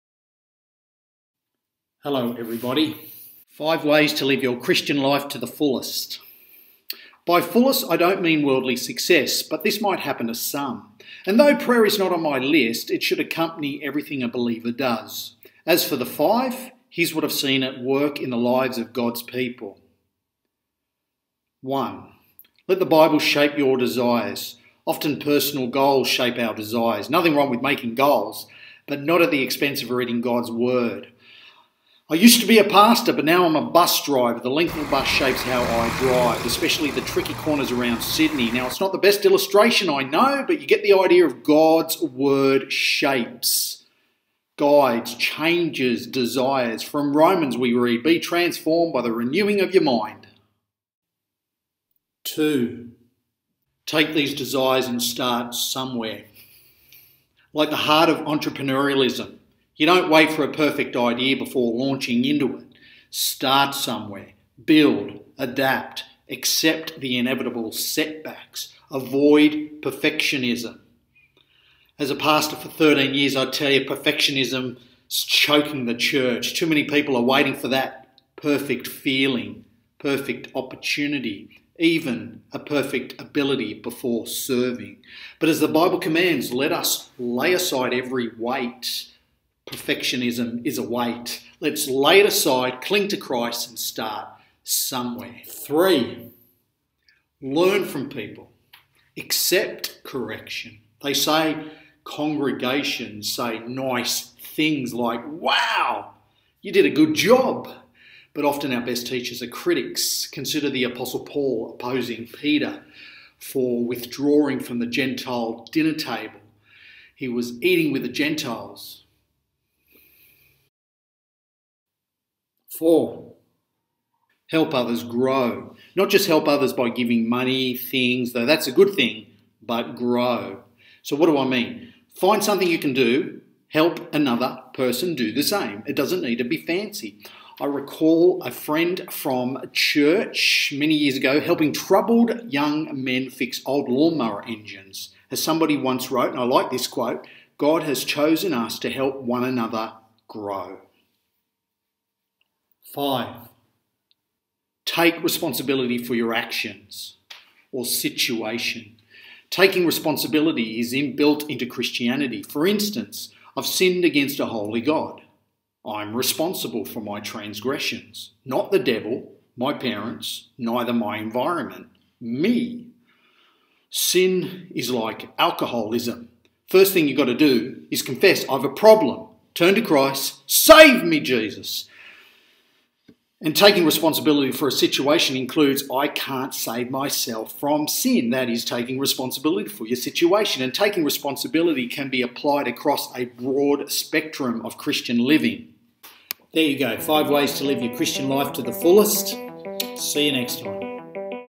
Life To The Fullest Christian
This video about 5 ways to live the Christian life to the fullest.